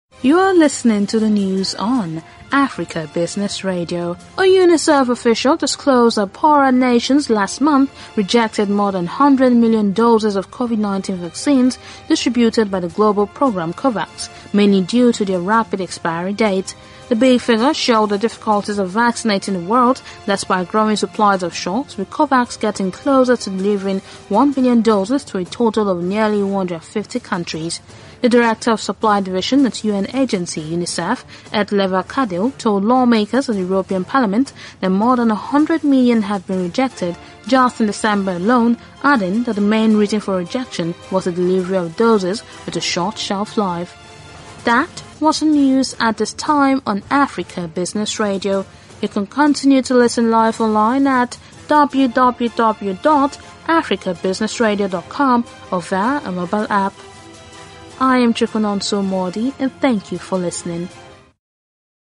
African Nations Reject COVID-19 Vaccines Over Short Shelf-life
A UNICEF official disclosed that Poorer nations last month rejected more than 100 million doses of COVID-19 vaccines distributed by the global programme COVAX, mainly due to their rapid expiry date.
The big figure showed the difficulties of vaccinating the world despite growing supplies of shots, with COVAX getting closer to delivering one billion doses to a total of nearly 150 countries.
The Director of Supply Division at UN agency, UNICEF, Etleva Kadil told lawmakers at the European Parliament that more than 100 million have been rejected just in December alone adding that the main reason for rejection was the delivery of doses with a short shelf-life.